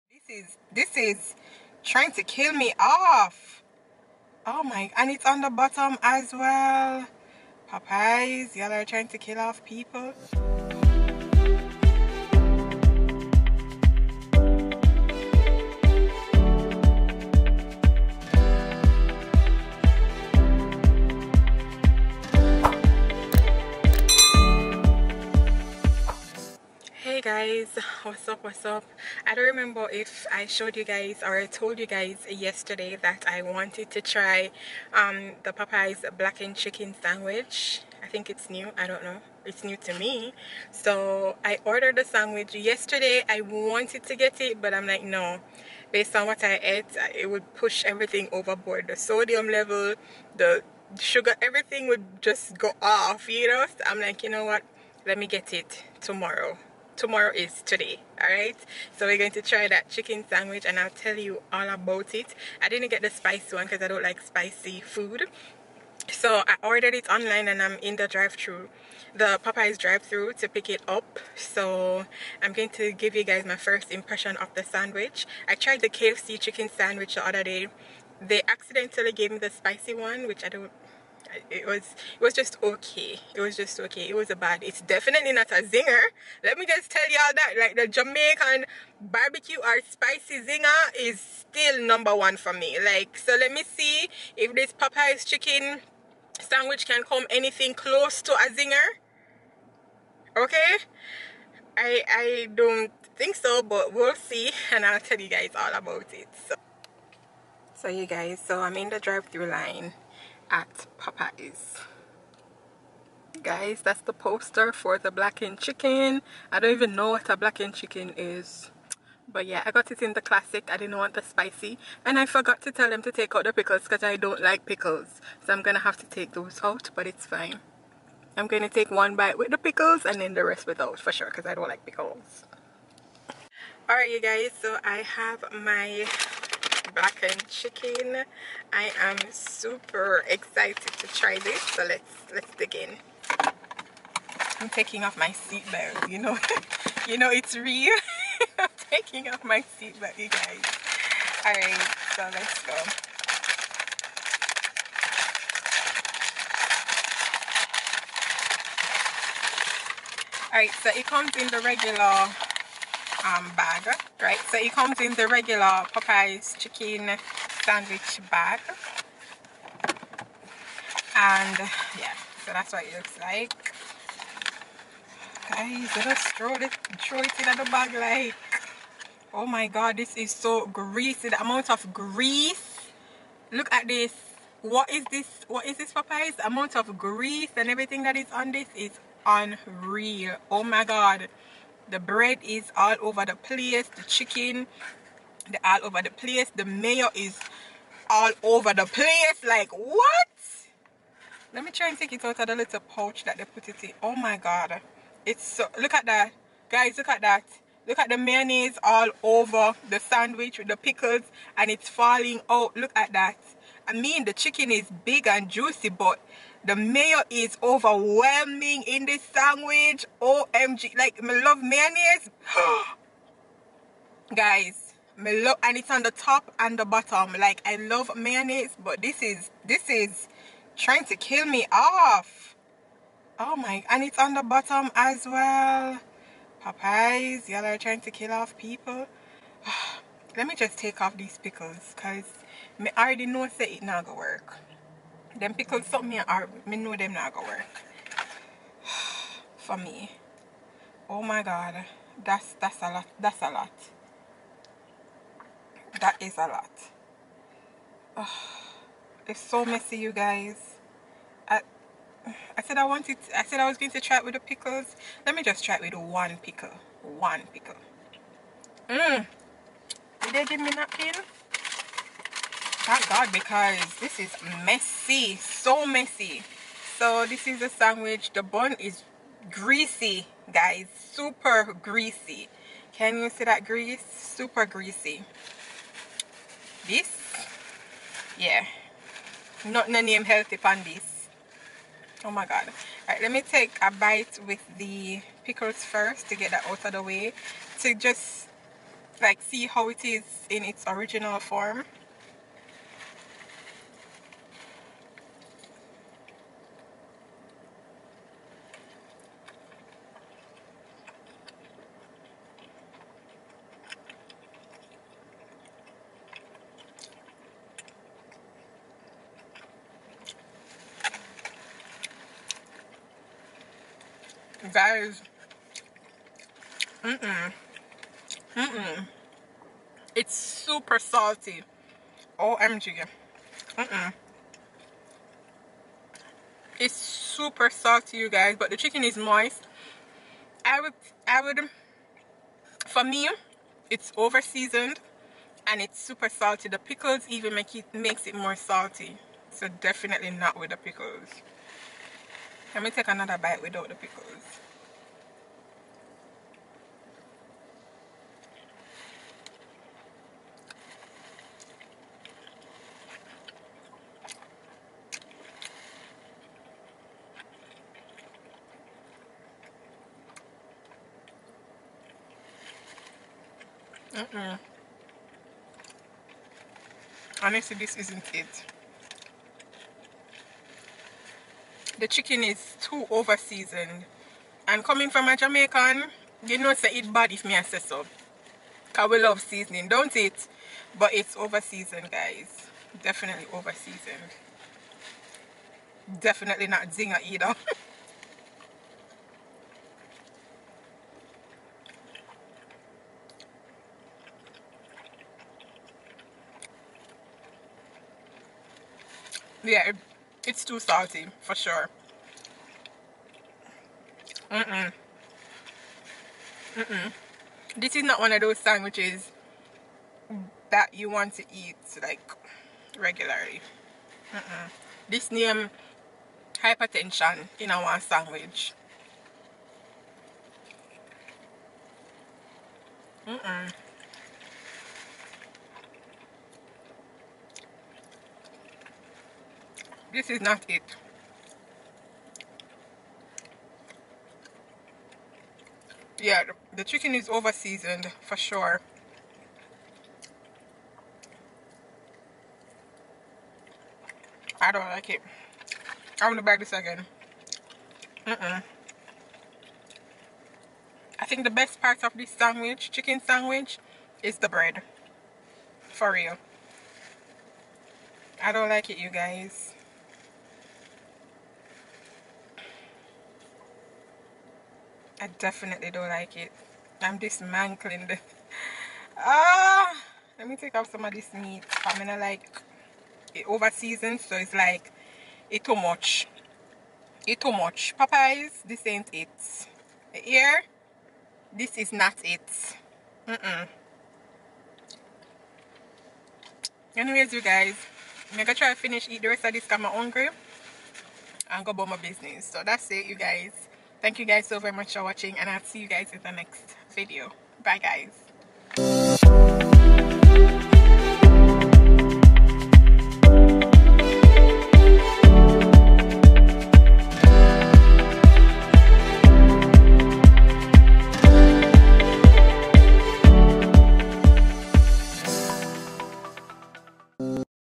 Popeyes NEW Blackened Chicken Sandwich Review | Sherica 💕
Popeyes bew Blackened chicken review!
Its giving. watch the video to find out 🤷🏾‍♀️

Sherica Love Ya!
●▬▬▬▬▬▬▬▬▬ஜ۩●۩ஜ▬▬▬▬▬▬▬▬▬●
Product List

Let me Beat your face/Work with me

●▬▬▬▬▬▬▬▬▬ஜ۩●۩ஜ▬▬▬▬▬▬▬▬▬●
This Video was created using:
Sony Alpha 5000 Camera